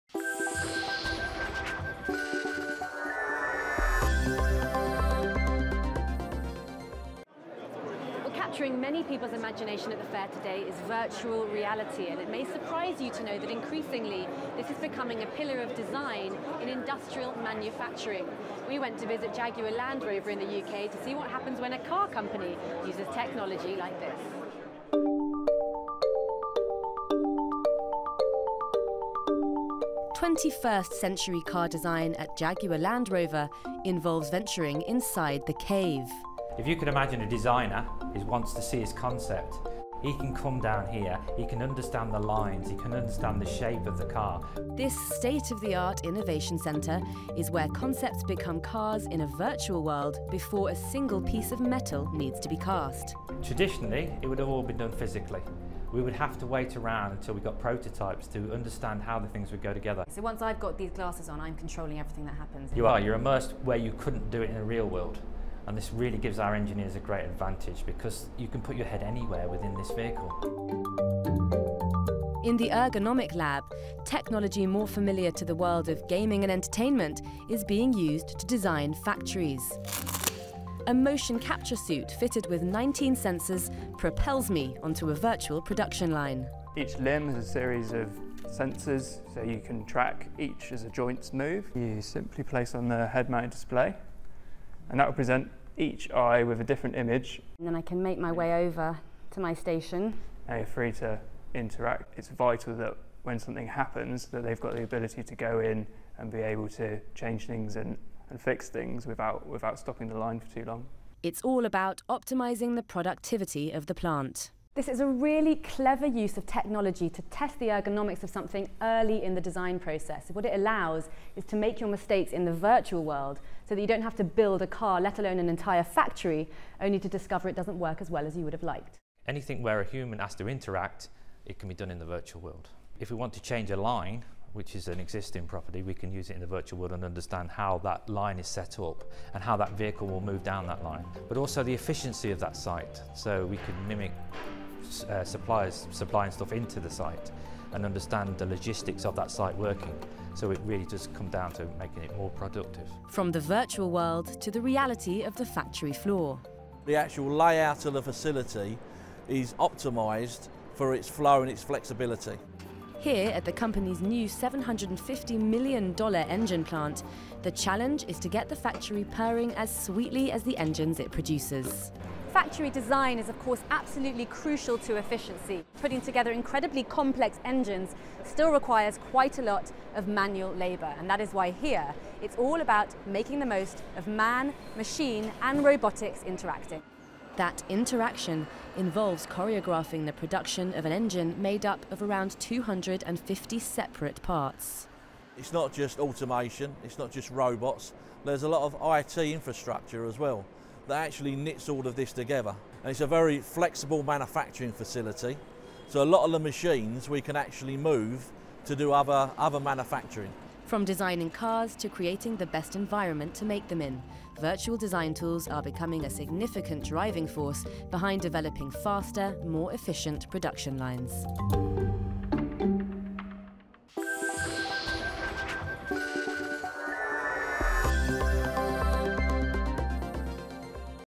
Inside Jaguar's Virtual Innovation Center
At this state-of-the-art innovation center, concepts become cars in a virtual world before a single piece of metal needs to be cast.